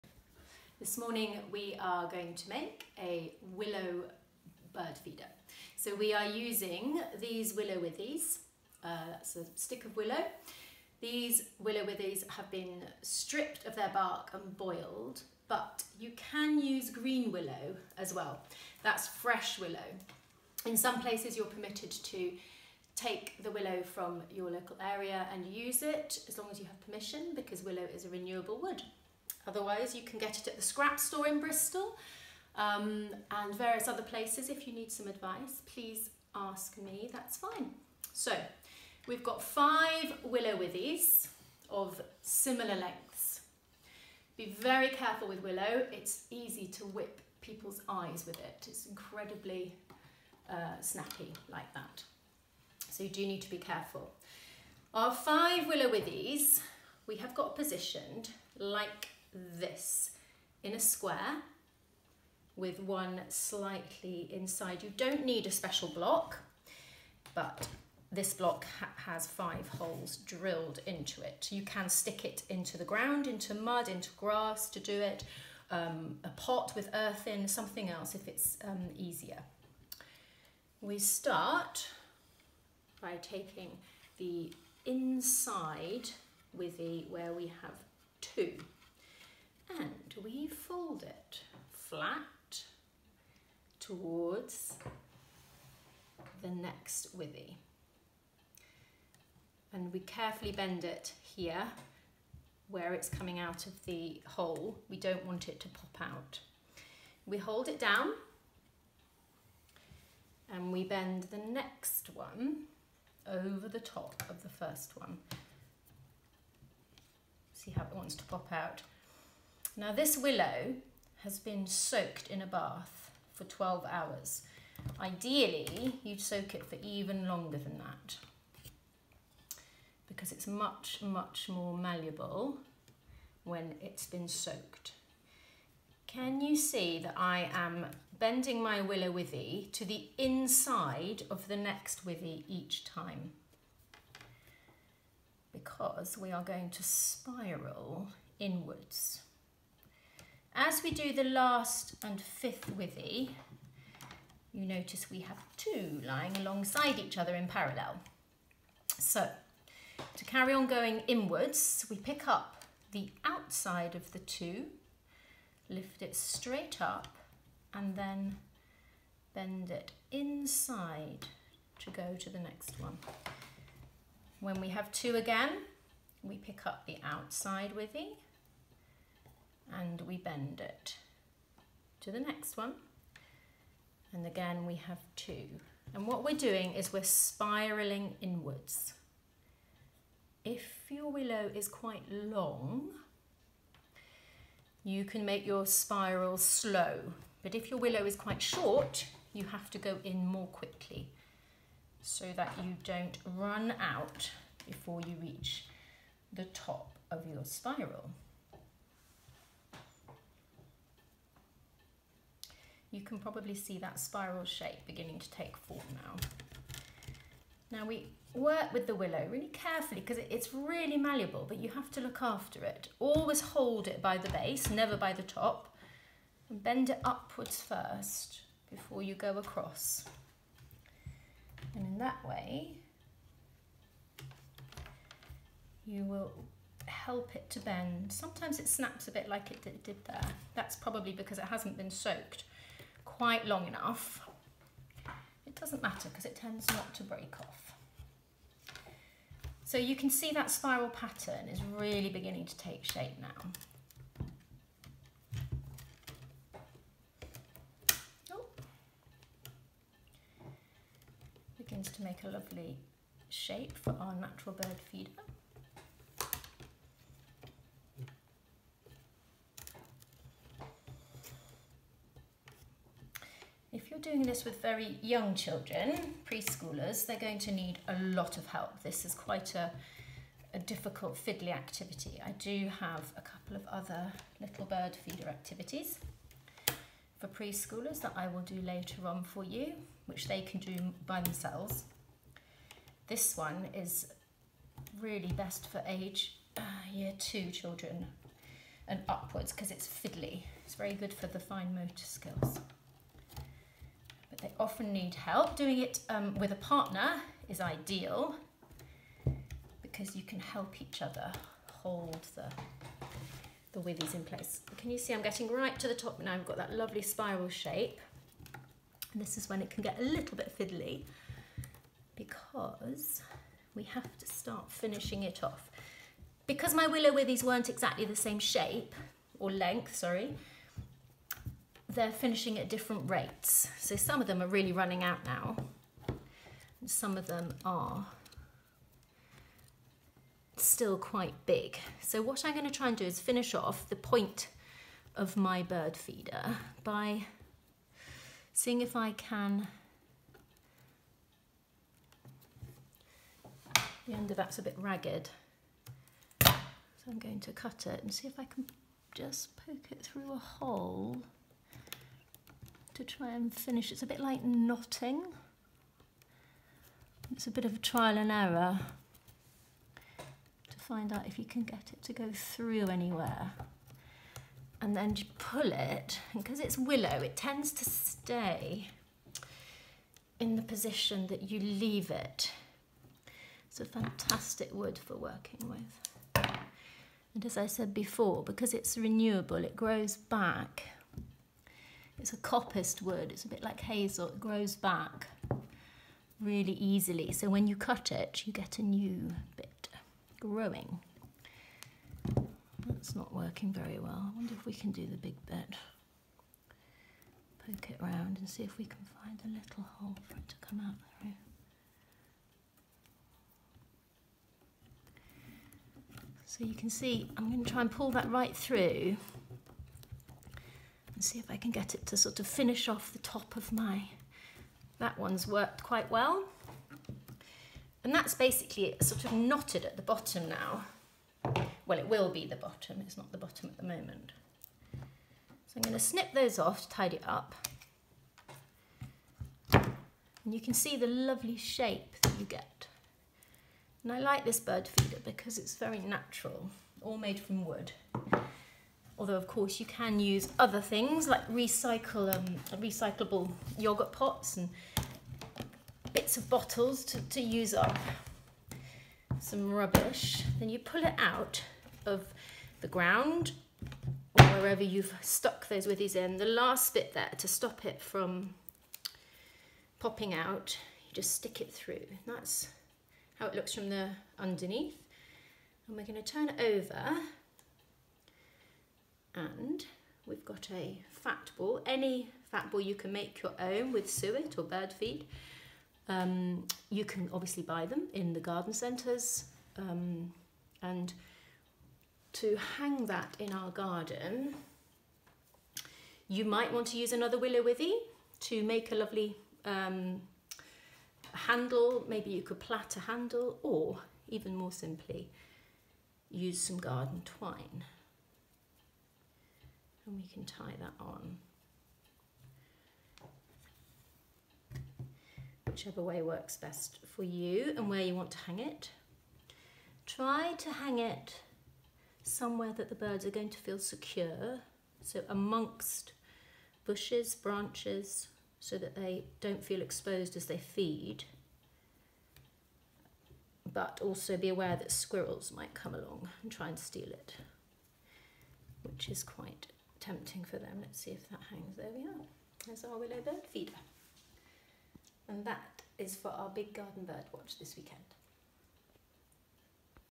Willow bird feeder lesson
Here we make a bird feeder out of willow withies to hang in your garden or near your home, during the Big Garden Birdwatch this weekend.